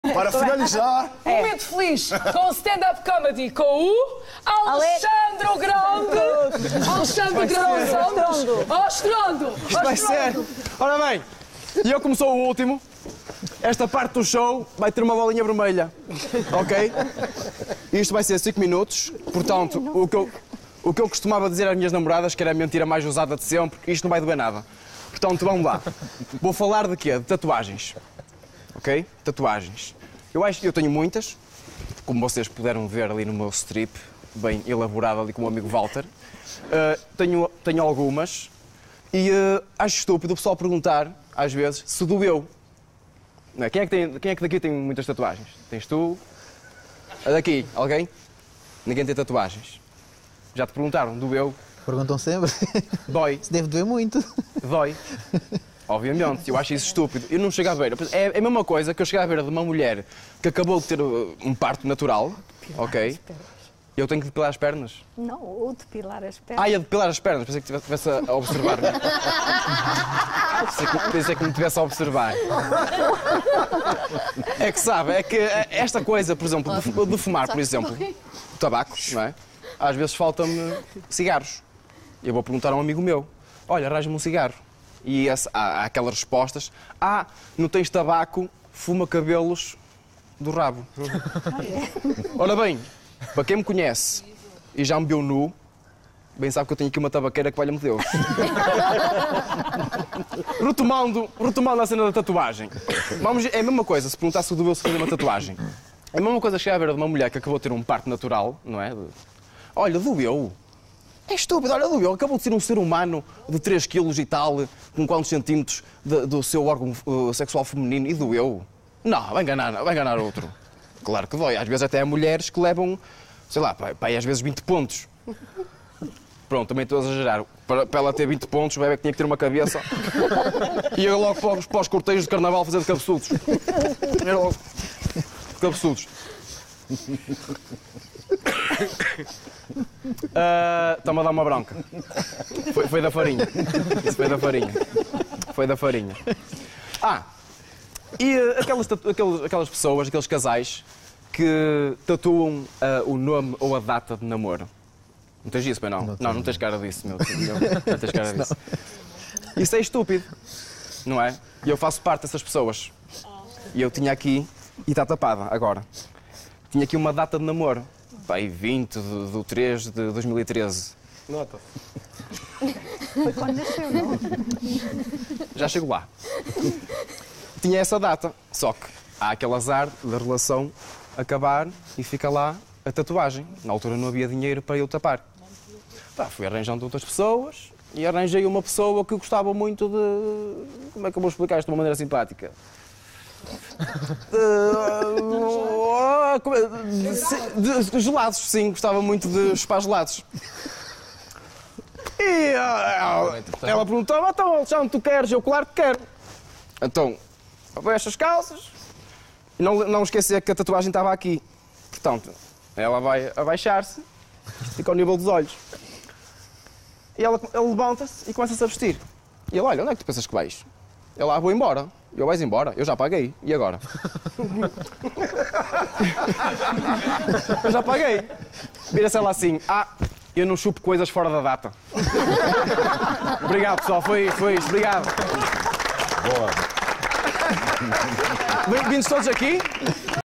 Alexandre Santos faz stand up comedy «picante»
Isabel Figueira introduz «Alexandre, o Grande», que rapidamente toma as rédeas do espetáculo. «Esta parte do show vai ter uma bolinha vermelha», promete Alexandre Santos, dando início a um momento de stand-up comedy. Alexandre Santos faz piadas «picantes» mas arranca aplausos de todos, no final.